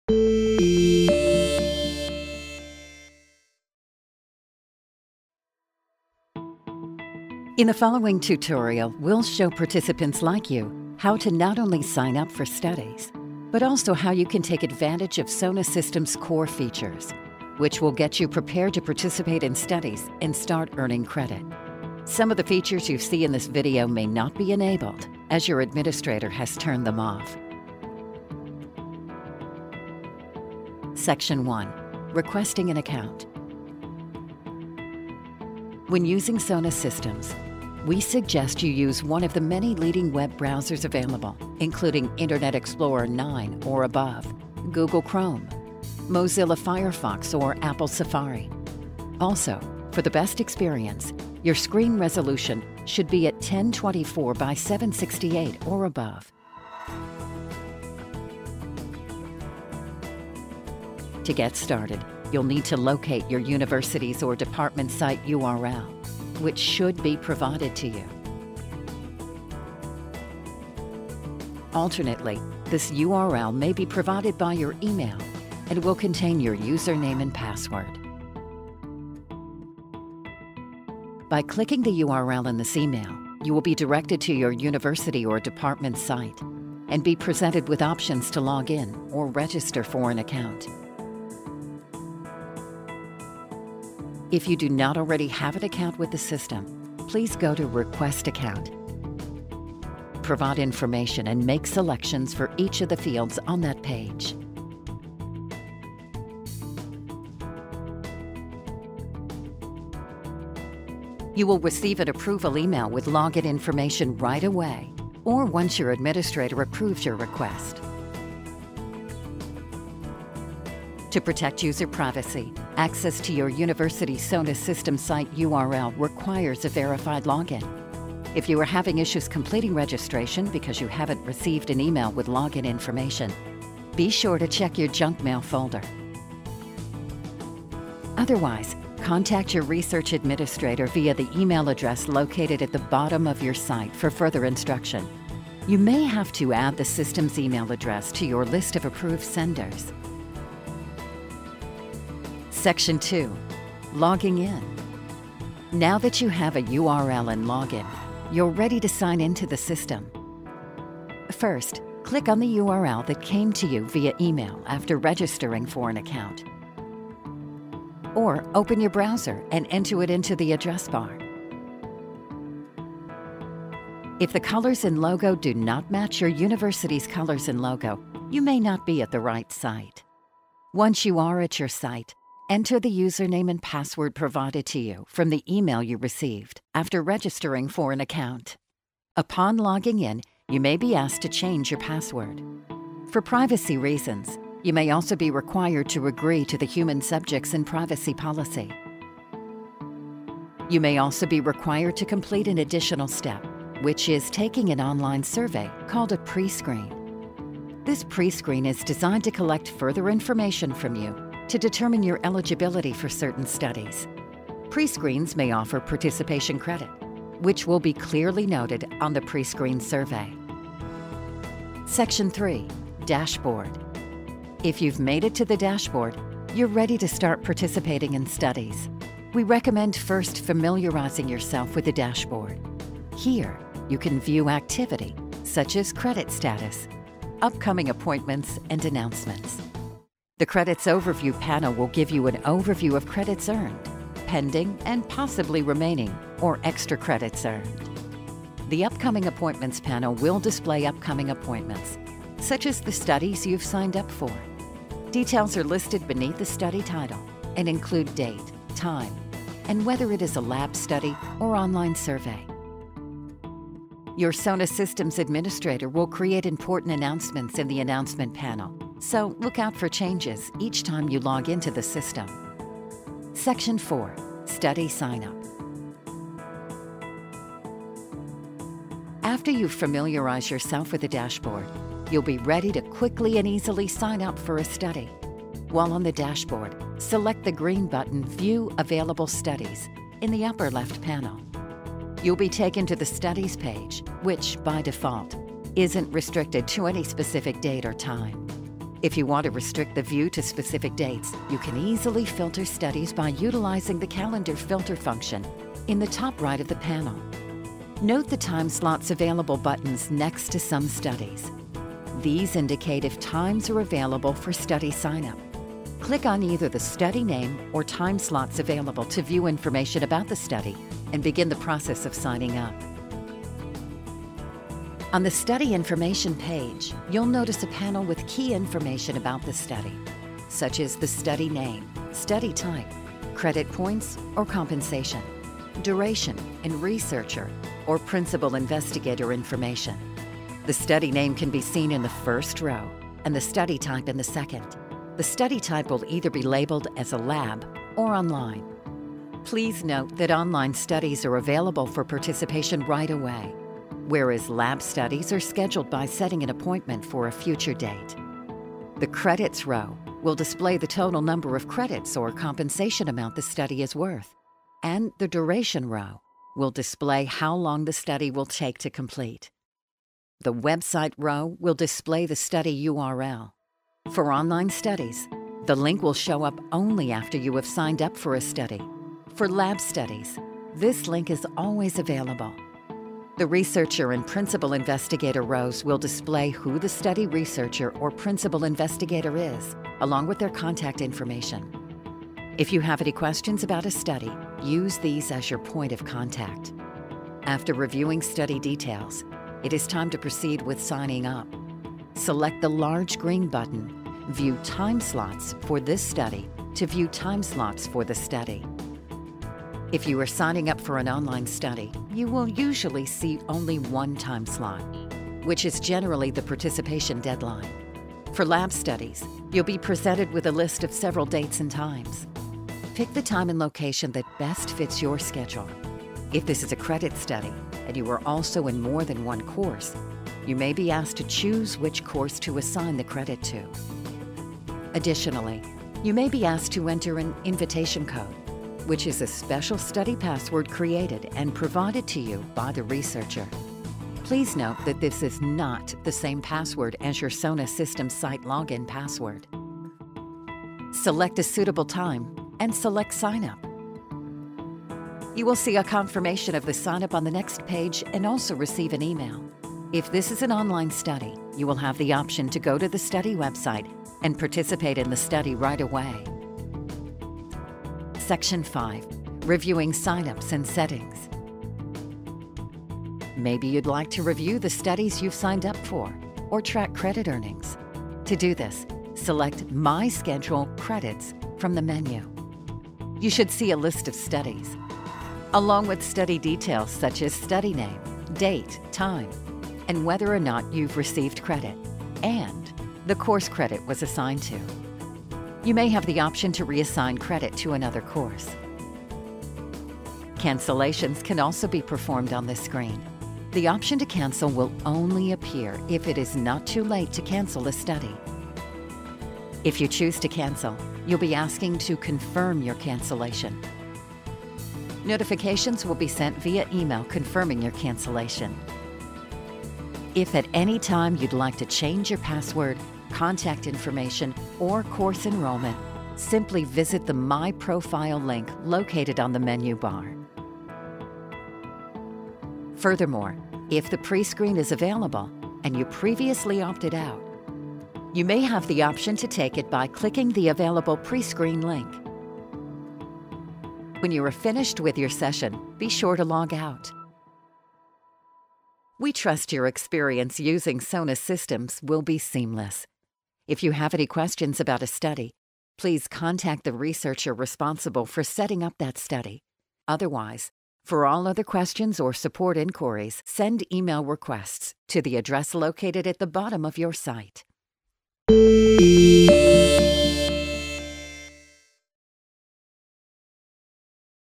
Sona Systems - Participant Tutorial
0:29 - SECTION 1: Requesting An Account
2:31 - SECTION 2: Logging In
3:41 - SECTION 3: Dashboard
4:33 - SECTION 4: Study Sign-up
7:47 - SECTION 5: Reviewing Sign Ups and Settings